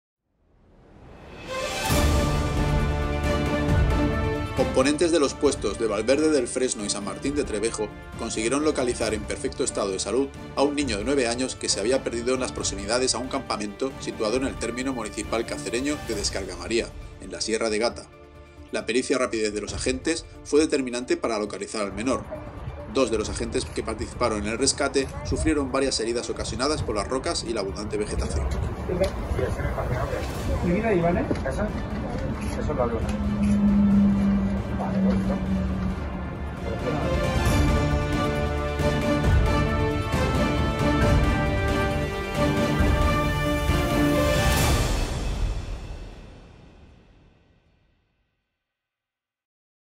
La Guardia Civil localiza en la Sierra de Gata, un niño de 9 años que había desaparecido
La pericia y rapidez de los agentes que intervinieron, fue determinante para la localización del menor

Dos de los agentes que participaron en el rescate, sufrieron varias heridas ocasionadas por las rocas y la abundante vegetación

09 de marzo de 2021.  Agentes de la Guardia Civil, pertenecientes a los Puestos de Valverde del Fresno y San Martín de Trevejo, consiguieron localizar en perfecto estado de salud, a un niño de 9 años, el cual se había perdido en las proximidades a un campamento situado en el término municipal cacereño de Descargamaría, en la Sierra de Gata.

LOS HECHOS

Sobre las 19,20 horas del pasado día 06 de marzo, la Central Operativa de la Comandancia de Cáceres, es alertada a través del 112, del extravío de un menor de 9 años de edad en la Sierra de Gata, concretamente en las proximidades a un campamento que existe en el término municipal de Descargamaría (Cáceres).

De forma inmediata, la Guardia Civil activa un dispositivo de búsqueda con diferentes patrullas, sobre todo por la hora del extravío, ya que empezaba a caer la noche y por las condiciones del terreno donde se había perdido.

Son las patrullas de los Puestos de San Martín de Trevejo y Valverde del Fresno, los que inician los primeros trabajos de rastreo y búsqueda en las proximidades al campamento, junto con los padres del menor, los cuales se encontraban pasando el día en dicho campamento.

En un primer momento, se inicia la búsqueda subiendo la montaña con el vehículo, para dejar el mismo en la parte alta con las luces prioritarias encendidas, con el objeto de que pudieran ser vistas por el niño y pudiera acudir al lugar, iniciándose la búsqueda a pie por la ladera de la montaña, llamando a viva voz al menor.

Habían transcurrido unos treinta minutos, cuando los agentes se percatan de un ruido entre la maleza, pensando que se pudiera tratar de un animal, dada la espesa vegetación y la oscuridad de la noche; tras prestar mayor atención, escuchan al niño llorando y pidiendo auxilio desde la parte baja de la montaña, en las proximidades a un arroyo, indicándole desde lo alto que no se moviera y permaneciera quieto en el lugar, dada la abundante vegetación, lo escarpado del terreno y el gran desnivel existente, siendo el acceso al lugar tanto para el niño como para los agentes muy peligroso.

Tras conseguir ubicarle con las linternas, los agentes deciden bajar hasta el lugar, produciéndose dos de ellos diversas lesiones como consecuencia de los salientes de las rocas y la abundante vegetación, consiguiendo finalmente localizar al menor, es cual a parte del estado de nerviosismo que presentaba y algunas pequeñas heridas, se encontraba en perfecto estado de salud, recibiendo los agentes de la Guardia Civil por parte del niño, un abrazo emocionado.

A partir de entonces, se inició el ascenso con el menor hasta donde se encontraba el vehículo y posterior traslado hasta el campamento, lugar donde se encontraban sus padres y varios familiares.

Destacar que en caso de pérdidas o extravíos en la montaña o en el campo, y siempre que se disponga de teléfono móvil, lo que se debe hacer de forma inmediata es llamar al 112, quien a través del teléfono nos ubicará en el punto donde nos encontramos, y a ser posible tener instalada en el móvil la aplicación ALERTCOPS, la cual en caso de pérdida nos situará en el punto exacto donde nos encontramos; además, es muy importante no moverse del lugar, ya que dificulta aún más la localización de la persona extraviada, intentando situarse en un punto alto.